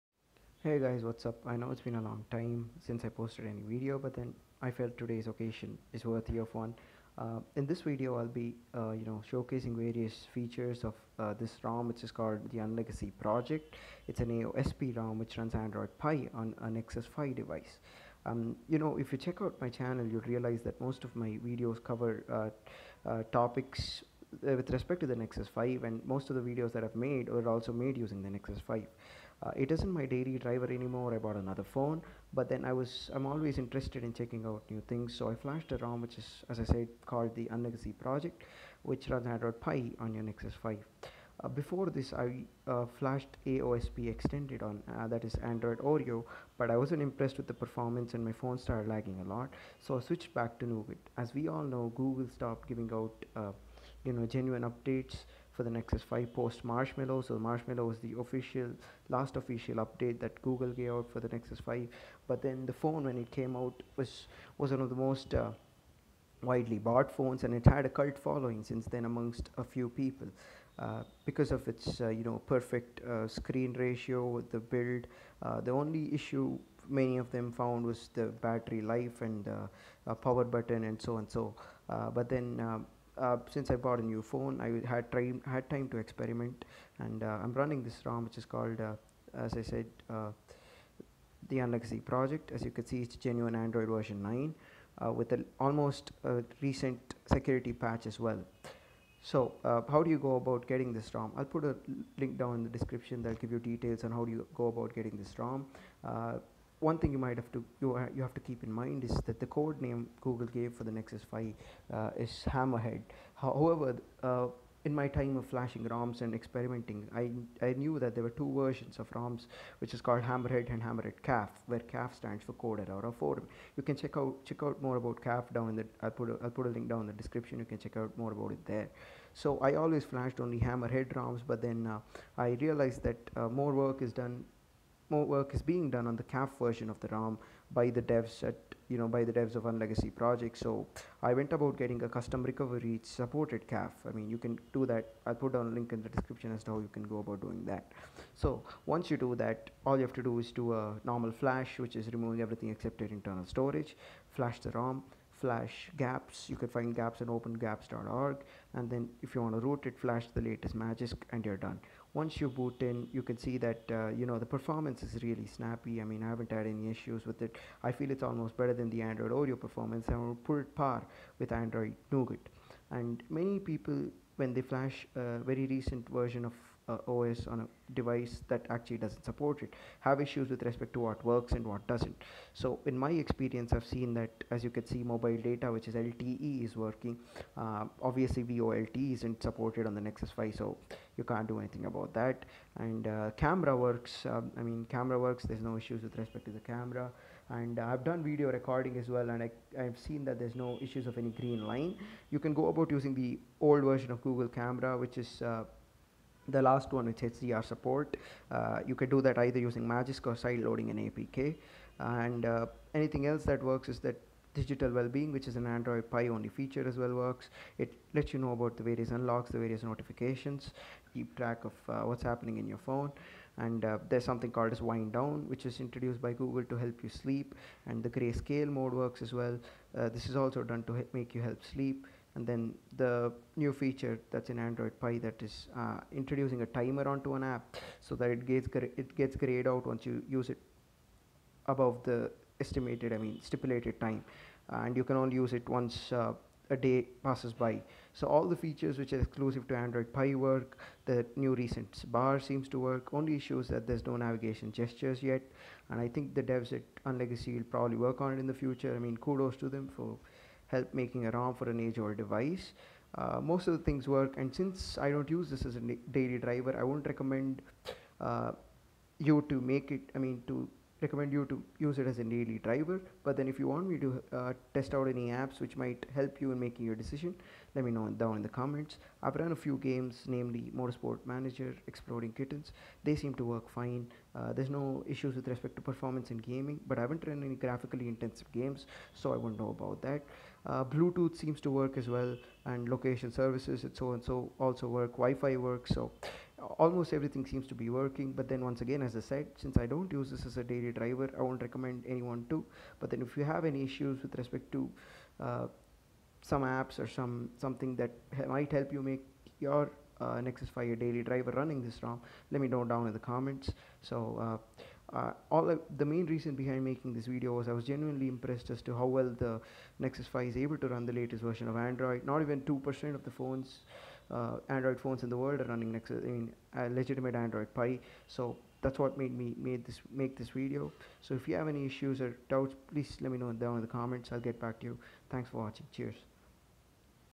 Android Pie On the Nexus 5 !
First up , I am sorry for mumbling a few words in the video was in a hurry but i hope the message i was trying to get across has been successfully delivered.
The unlegacy project Nexus 5 links are on the second comment / post. They link to a box folder .

Gapps - I personally use the pico version but u can use the one that suits your requirements .

I haven't personally tested this version of google camera on the pie rom but i have used it before and HDR works and the pictures are better!

If u any issues or queries lemme know in the comments.